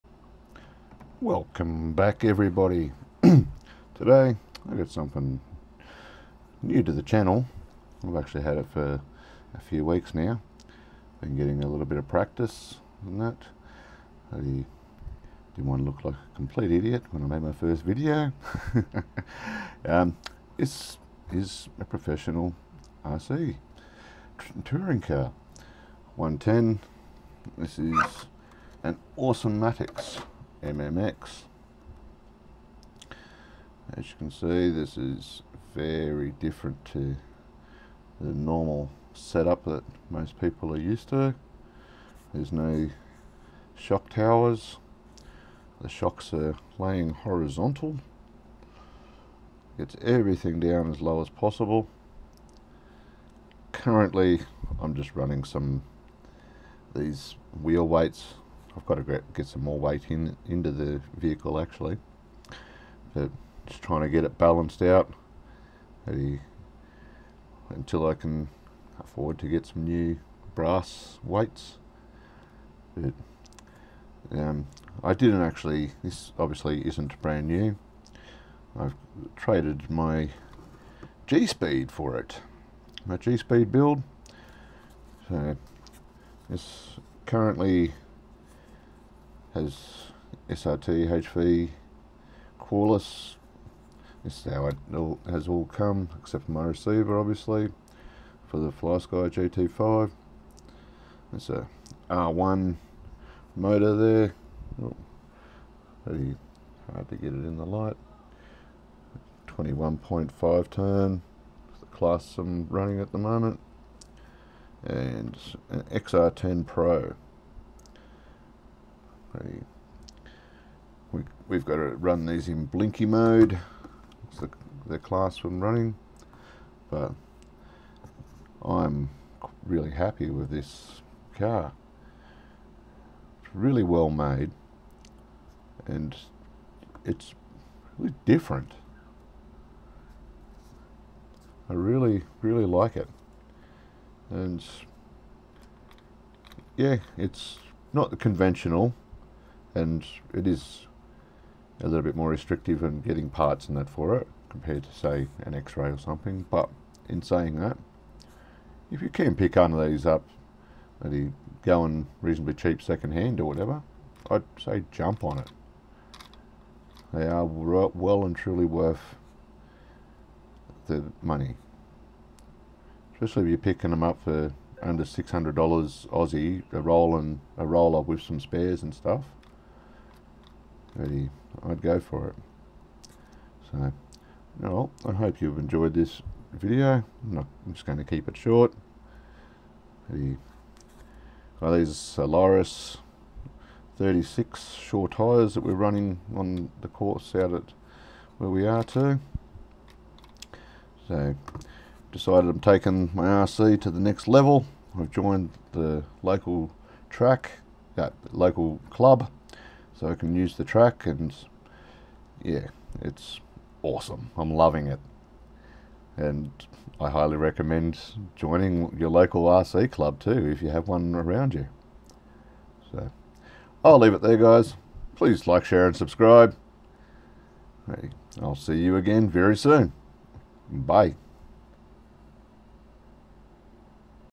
Awesomatix A800 MMX Pro RC Touring car. Taking RC to the next level.
Today im showing my Awesomatix MMX Pro RC touring car which i traded my Gspeed build for and I Joined my local RC club.